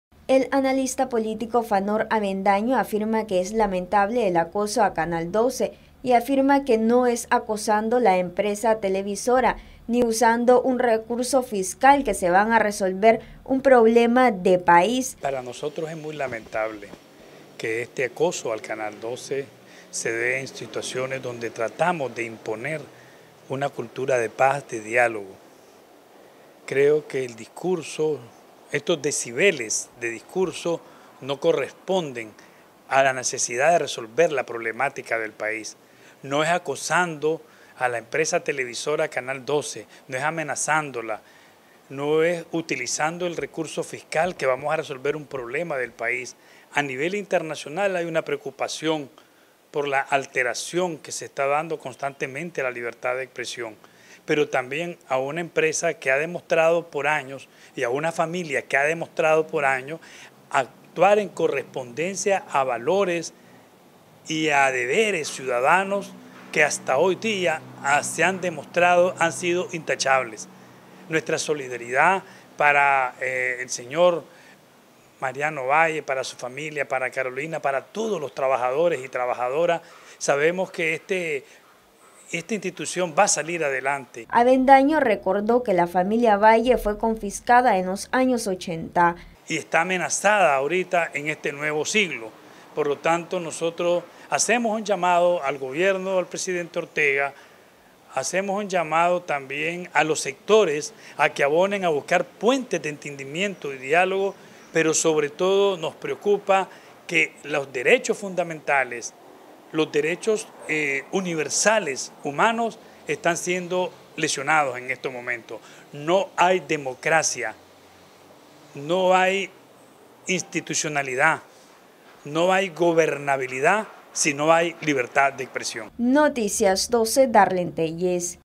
Preocupa a nivel internacional embargo a Canal 12
Analista político Fanor Avendaño manifestó que es lamentable el acoso a canal 12 y se solidarizó con la familia Valle y sus colaboradores por la situación que atraviesan.